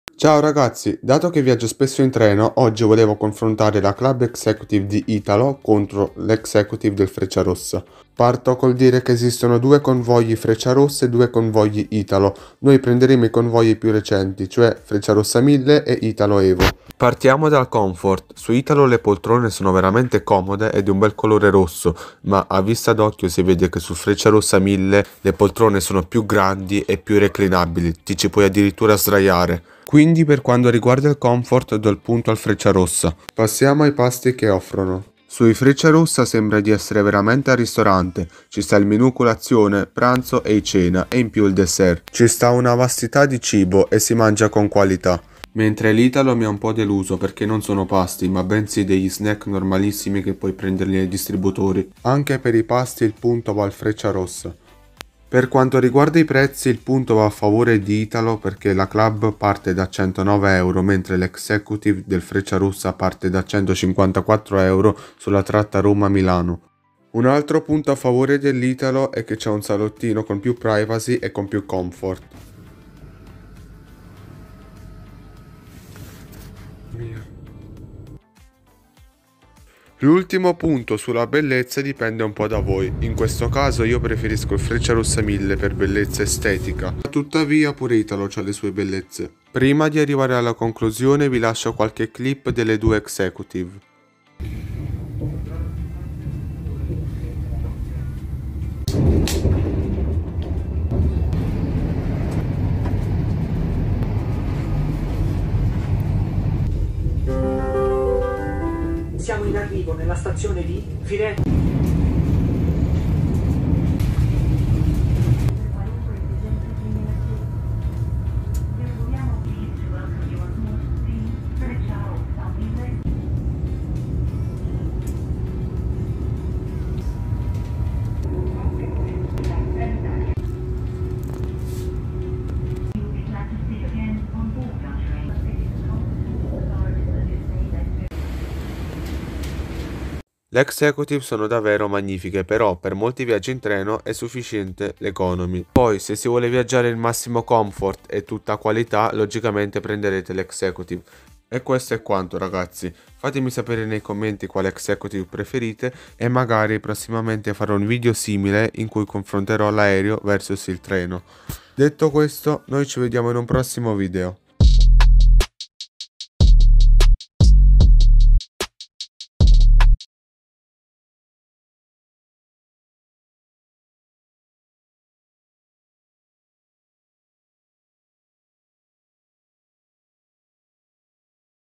Executive frecciarossa vs Executive italo
◾EXECUTIVE frecciarossa vs italo◾
0:18 Confort Italo
0:22 Confort Frecciarossa 1000
0:33 Pasti frecciarossa 1000
0:46 Pasti di italo
0:55 Prezzi
1:07 Salottino di italo
1:19 Bellezza estetica
2:20 Conclusione